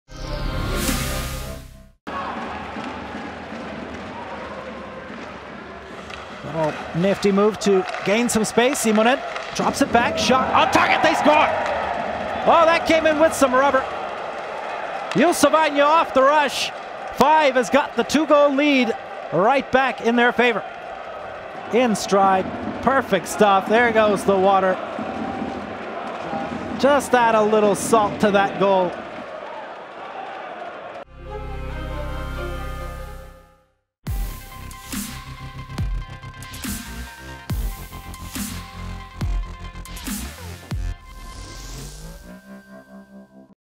Juuso Vainio - JYP Jyväskylä | Top Goal
Goal of the Season Voting - Top Goal by Juuso Vainio (JYP Jyväskylä) against Oceláři Třinec at the Semi-Final Return Game.